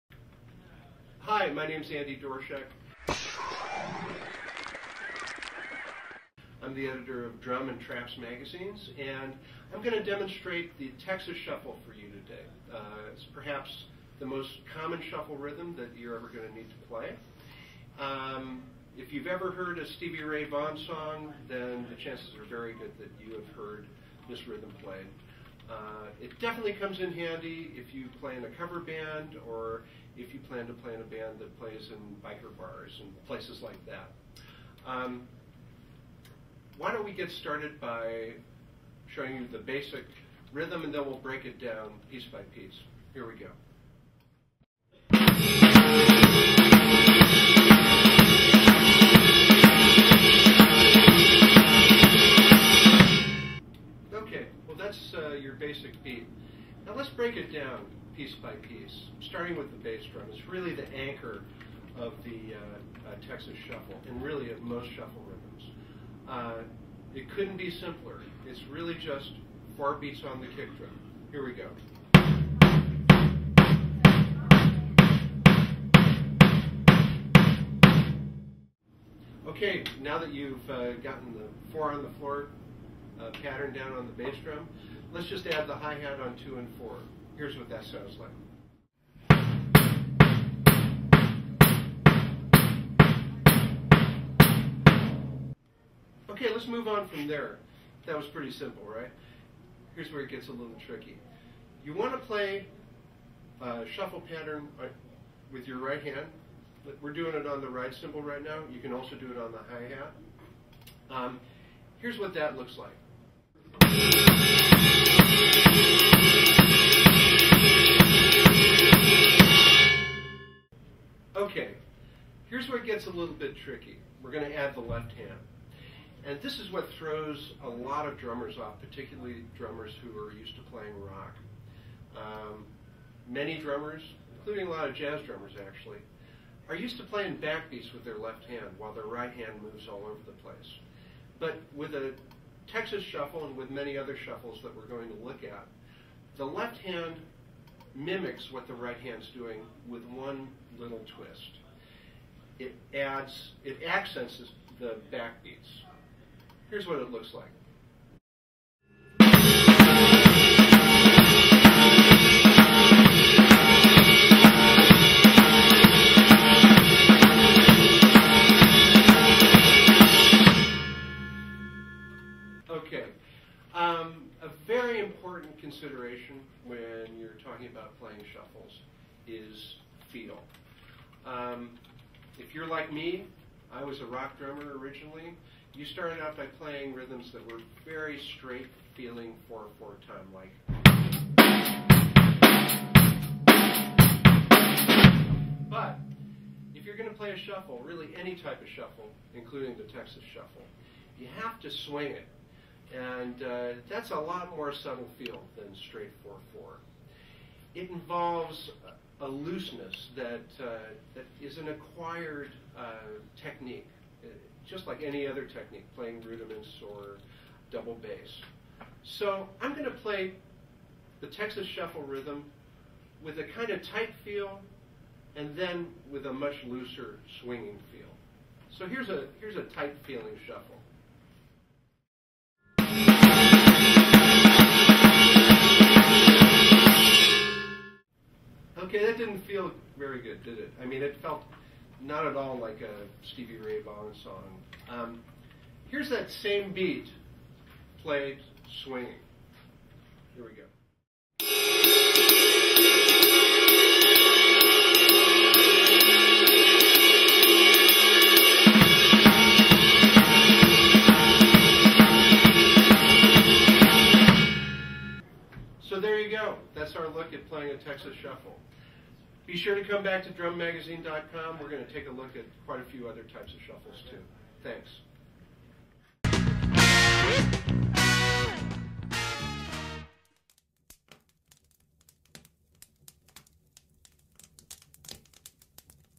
Drum Lesson: How to Play a Texas Shuffle
Get Your Free DRUM! Mag Pack DRUM! Founding Editor Andy Doerschuk breaks down a Texas shuffle on the drum set.